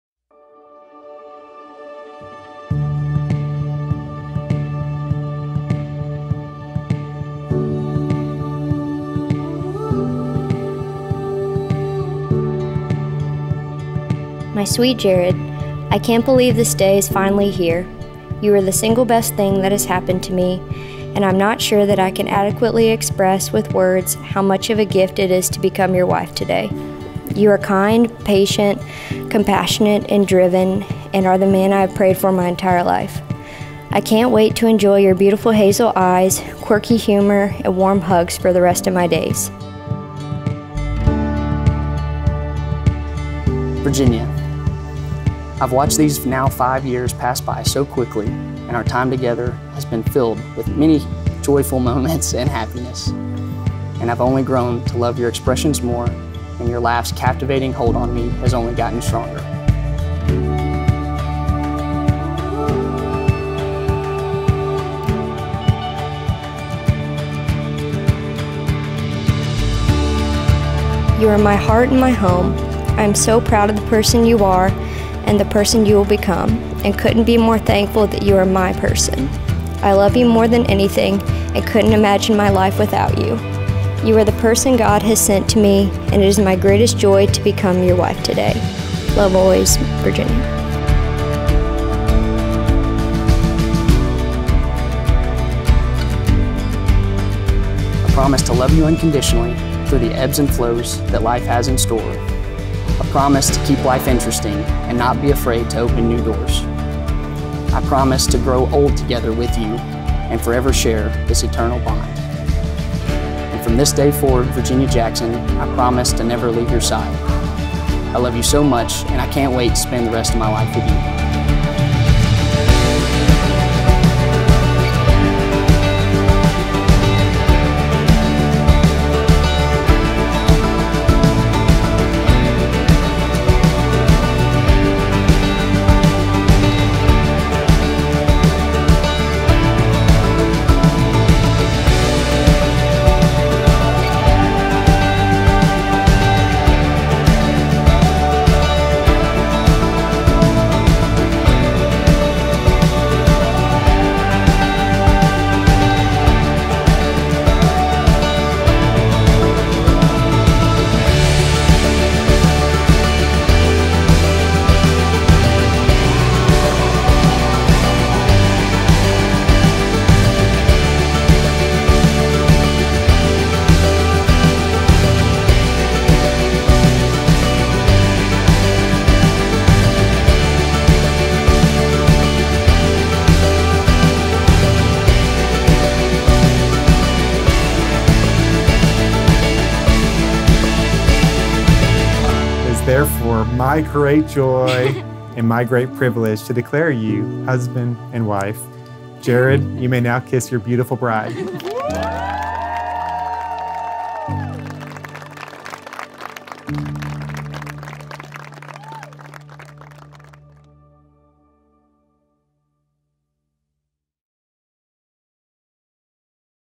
Jarrod and Virginia at Sycamore Farms
We're so excited to share Jarrod and Virginia's wedding highlight film! They had a beautiful day out at Sycamore Farms surrounded by family and friends. Plus, Jarrod works on rockets. How cool is that? We were thrilled to be able to capture their big day!